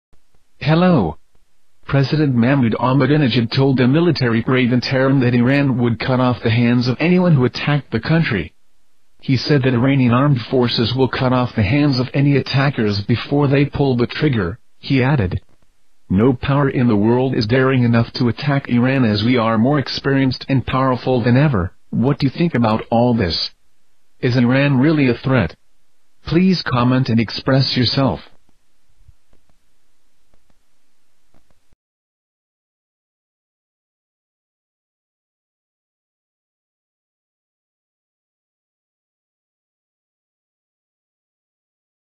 Iran's President Will Cut Your Hand Off If You Don't Behave
President Mahmoud Ahmadinejad told a military parade in Tehran on Tuesday Iran would "cut off the hands" of anyone who attacked the Iran.